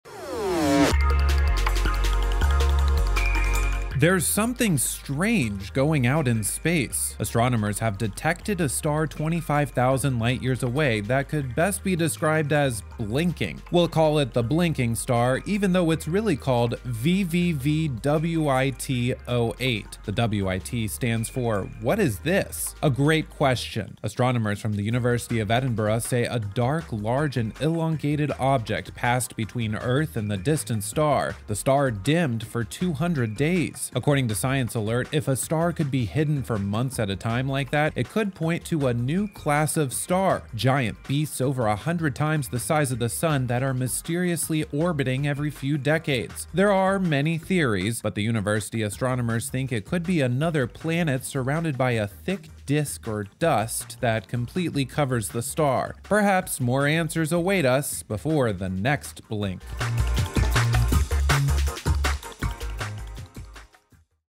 What is that Mysterious ‘Blinking’ Object in Space?
Astronomers have detected a star 25,000 light-years away that can best be described as “blinking.”